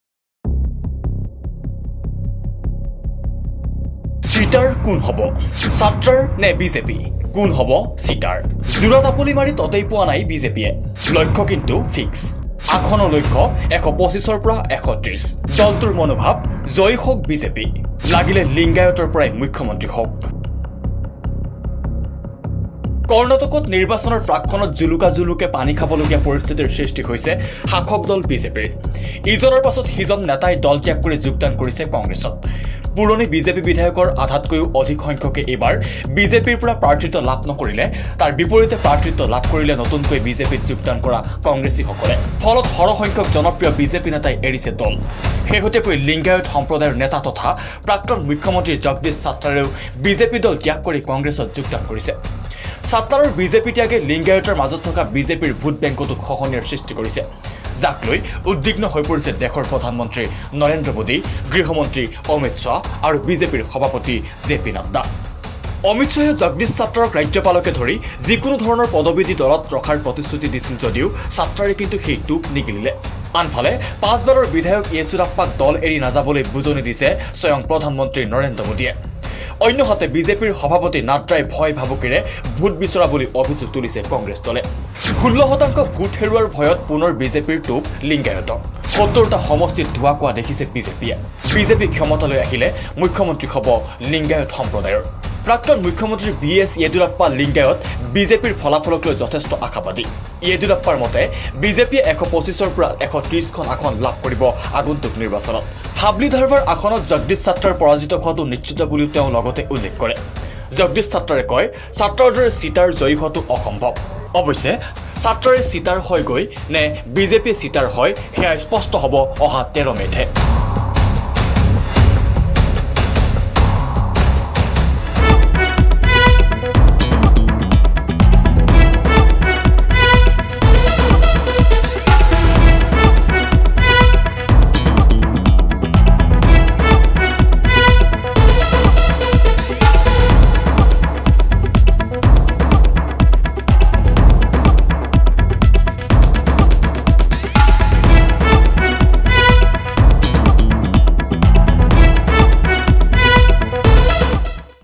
কৰ্ণাটকত নিৰ্বাচনৰ প্ৰাক ক্ষণত জোলোকা জোলোকে পানী খাবলগীয়া পৰিস্থিতি শাসক দল বিজেপিৰ
কৰ্ণাটকত নিৰ্বাচনৰ প্ৰাক ক্ষণত জোলোকা জোলোকে পানী খাবলগীয়া পৰিস্থিতি শাসক দল বিজেপিৰ৷ ইজনৰ পিছত সিজন নেতাই দল ত্যাগ কৰি যোগদান কৰিছে কংগ্ৰেছত